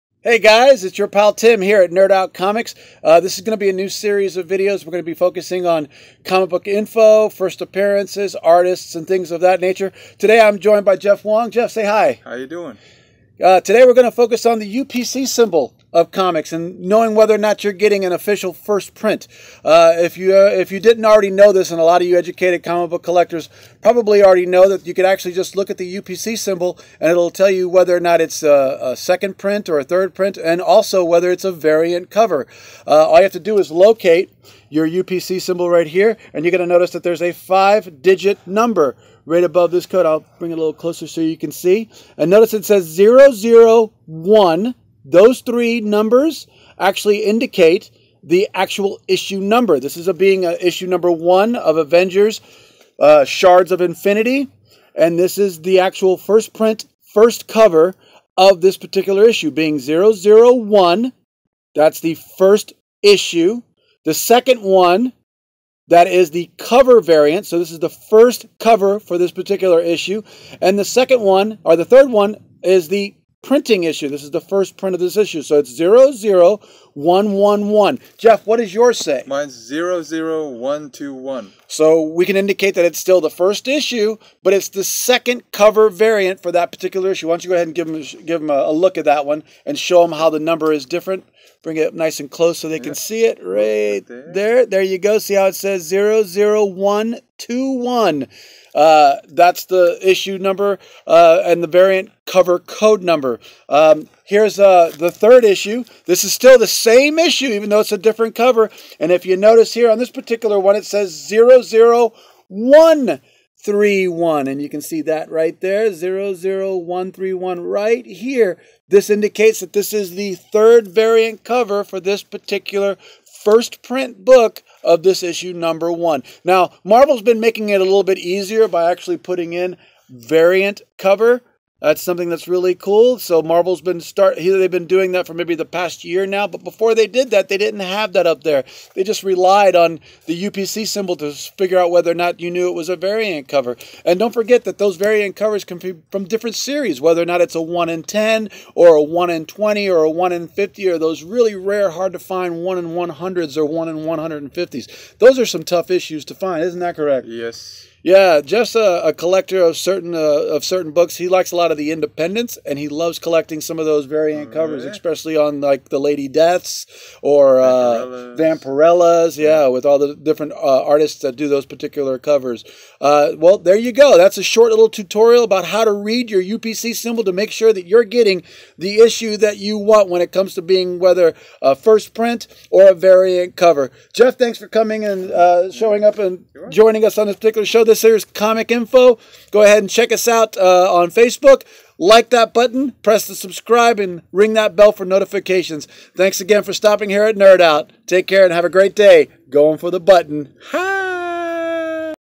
Comic UPC codes!
Wanna know how to decipher those UPS codes for true first prints?? Well here ya go!